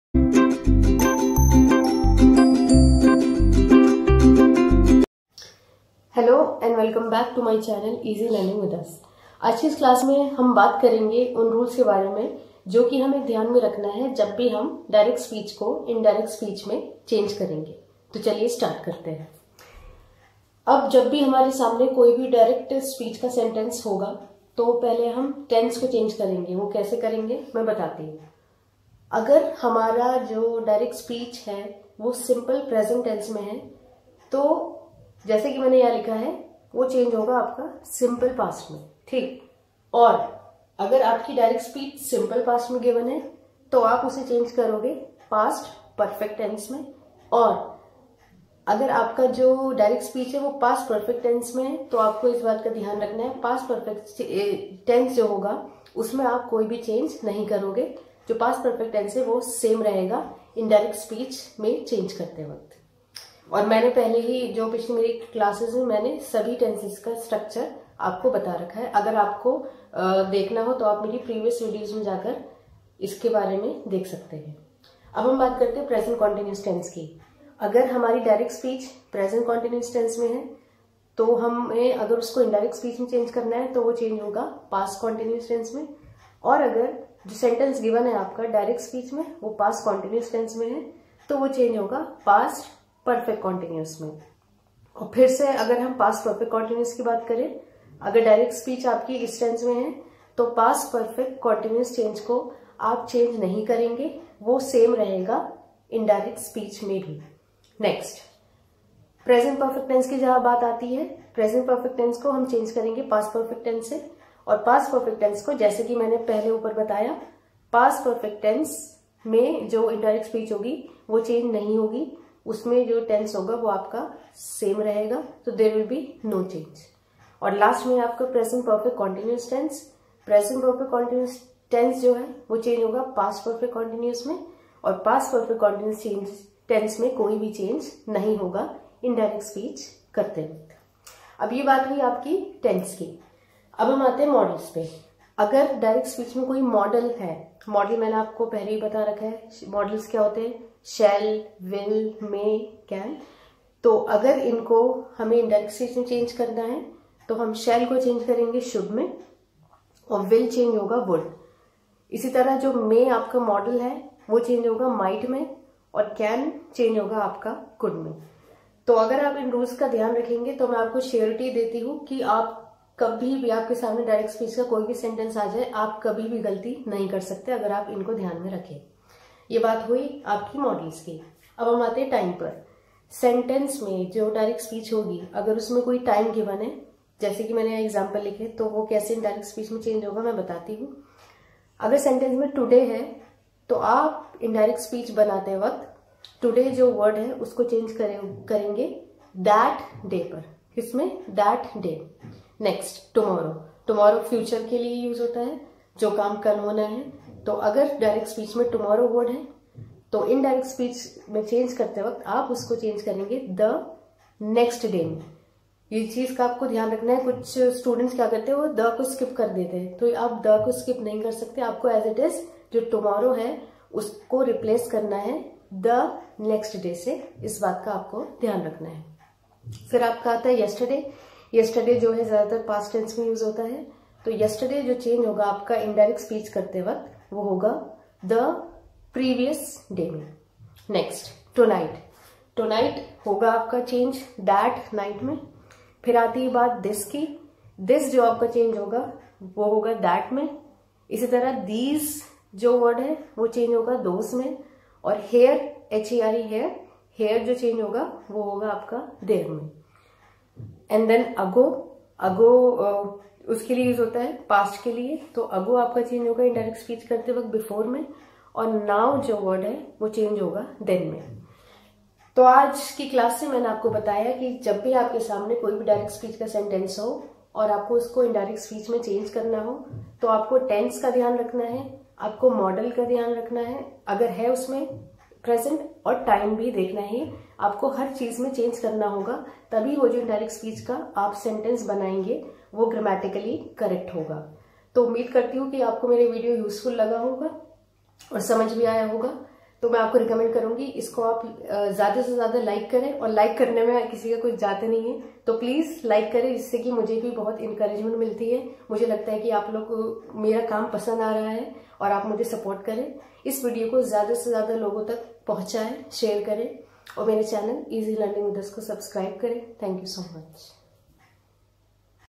Some More Rules of Reported Speech
This video explains about some more rules which are required to change direct speech into indirect speech.

This is useful for the students of all levels who are learning English language or preparing for any kind of competitive exams.

Keep supporting us with your valuable comments and love!